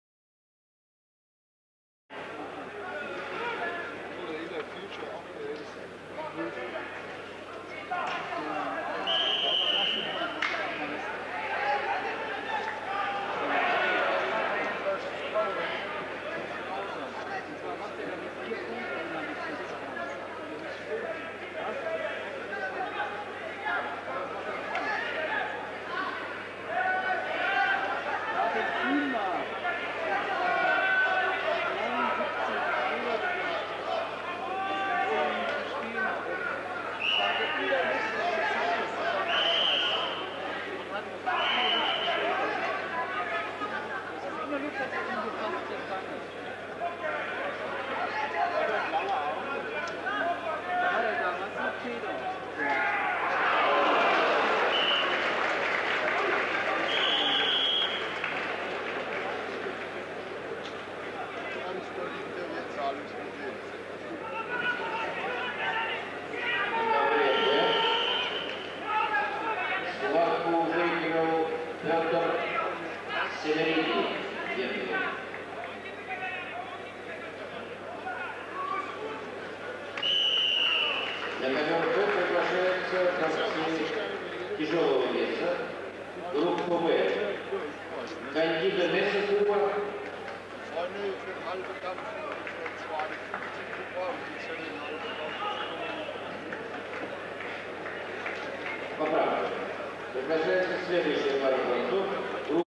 1983 Senior World Championships: 90 kg Akira Ohta (JPN) vs. Jan Gorski (POL)
1983 Senior World Championships 90 kg Match Between Akira Ohta (Japan) and Jan Gorski (Poland)